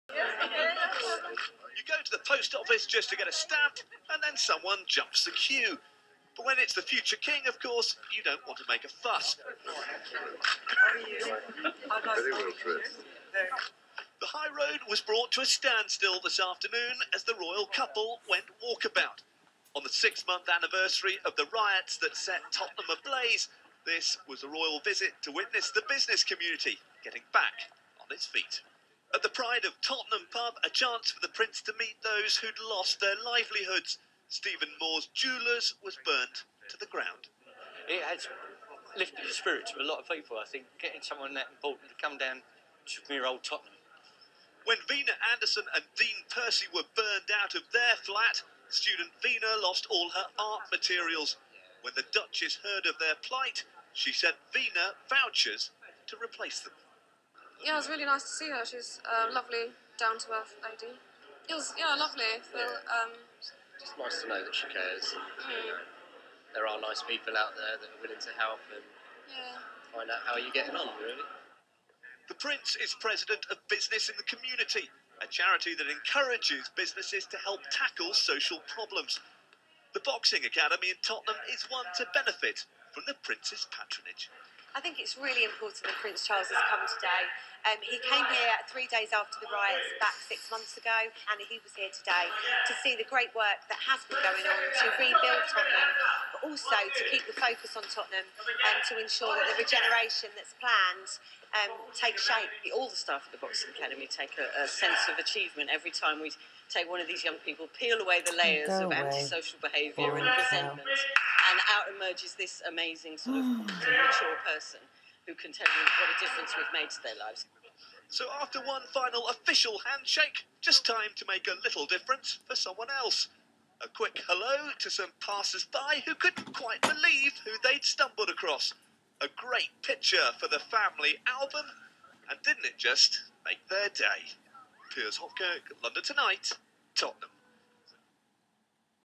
Prince Charles visits post-riots Tottenham 09/02/2012
February 9th 2012. Royal visit to Tottenham. TV interviews in the Boxing Academy. Pod 3 in all their glory!